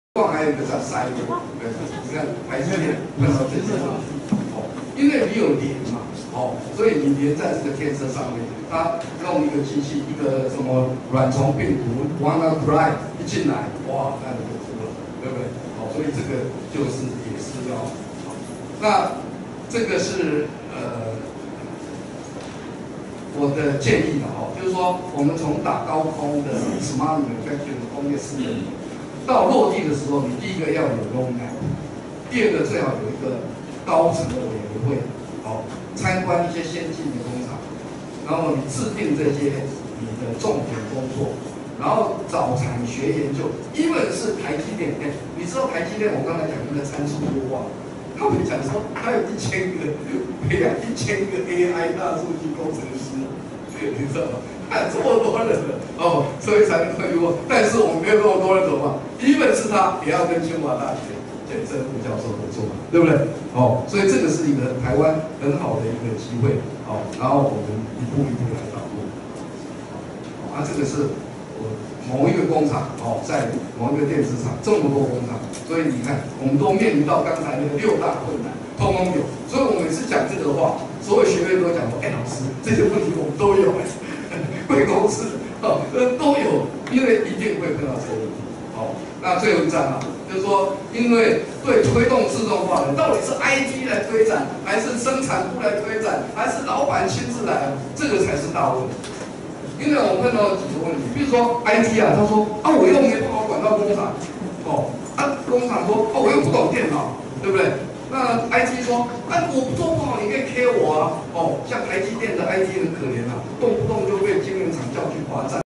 20190317 瑞士歐洲大學_企業接班、創新與轉型論壇_智慧製造科技與應用 李良猷總裁主講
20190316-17 企業接班、創新與轉型論壇(歡迎索取精華版講師授課投影片)
主辦單位：(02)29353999 中歐領袖學院高等教育中心/大人物知識管理集團/瑞士歐洲大學

生產力4.0系統架構是透過物聯網將生產資訊數位化，延伸應用及管理後，達成聯網服務製造系統之創新營運模式。
區塊鏈若要應用在工業4.0，生產履歷、智慧合約、供應鏈及聯網設備等該如何管理？
雙偉通聯亞太區總裁李良猷為您深入解說智慧製造科技與應用！